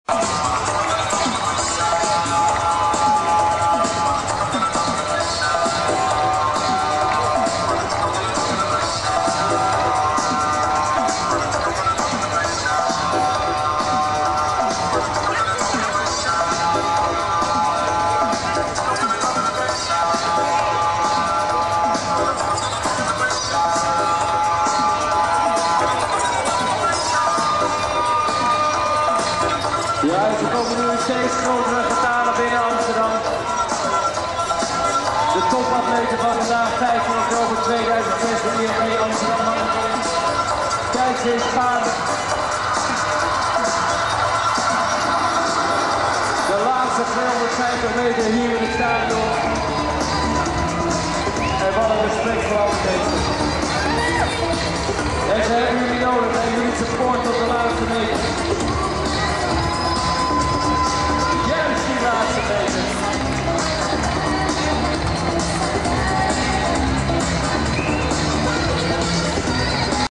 Amsterdam Marathon - Finish Line
Yael and I can be spotted crossing the line after 52 seconds!